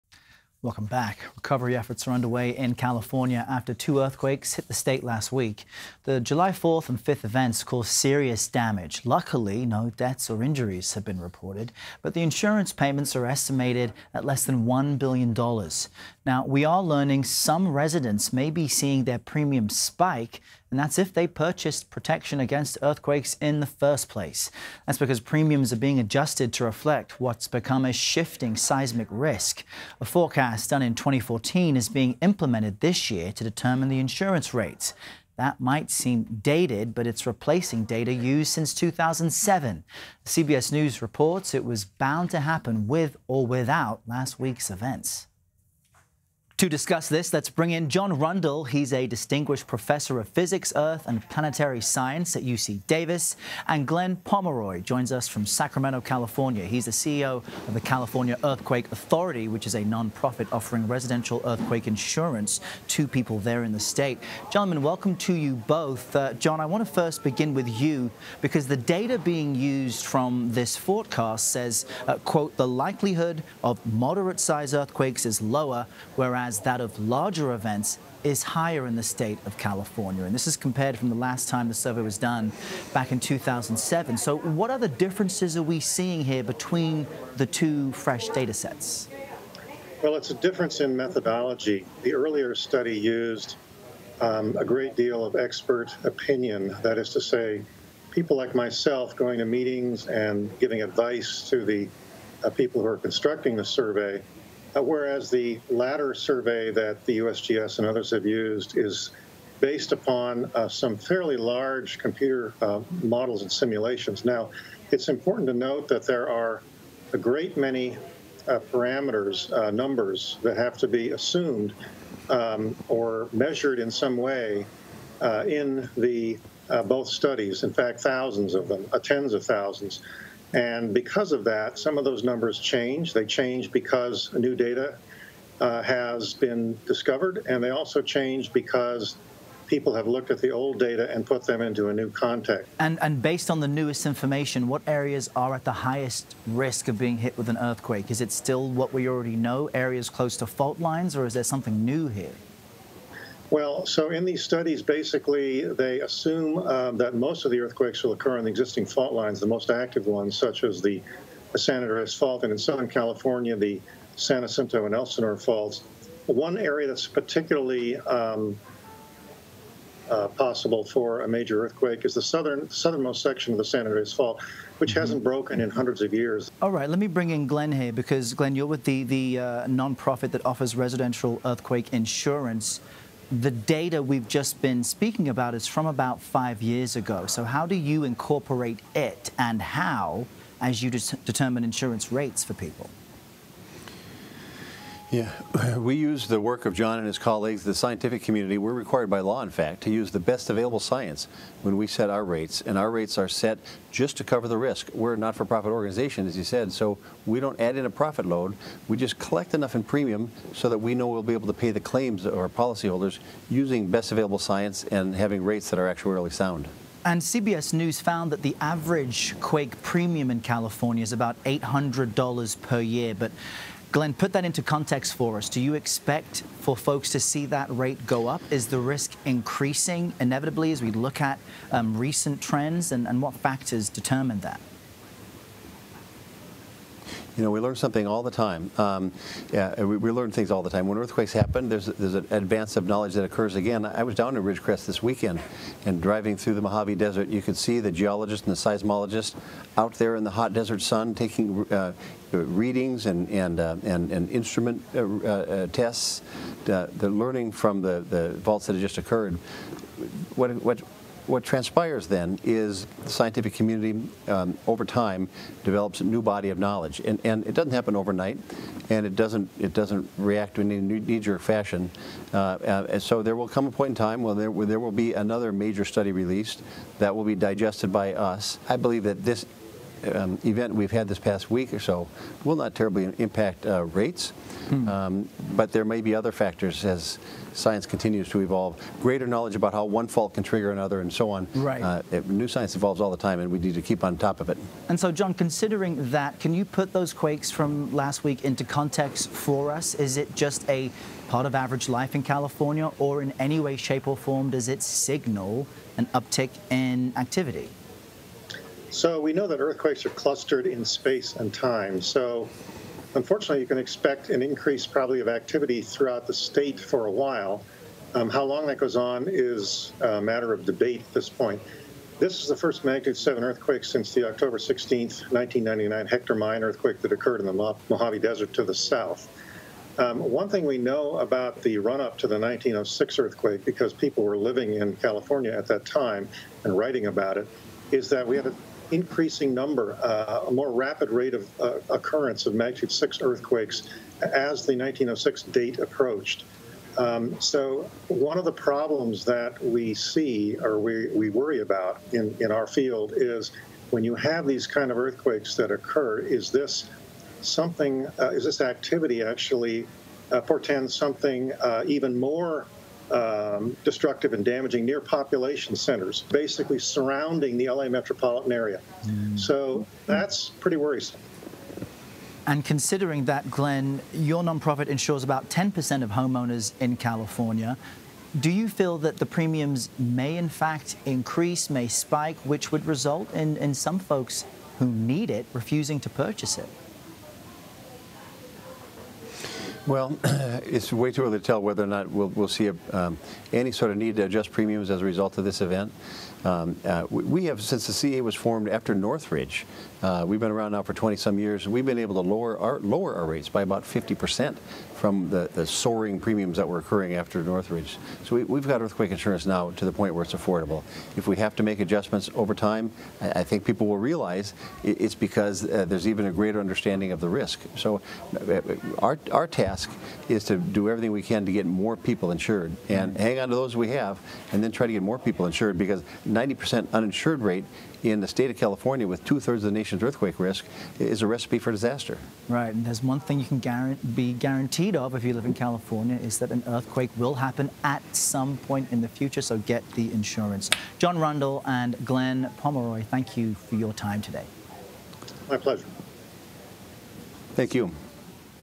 Earthquake insurance rates to soar for many Californians
Earthquake insurance rates are expected to spike for some residents in California. John Rundle, a distinguished professor of physics, earth and planetary science at University of California - Davis, and Glenn Pomeroy, CEO of the California Earthquake Authority, join CBSN to discuss.